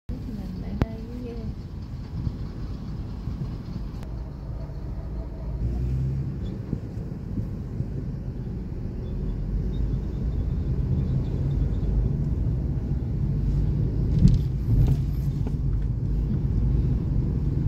Xuống đường 'phá băng' bất động sản
Phát tờ rơi bán bất động sản bên rừng cao su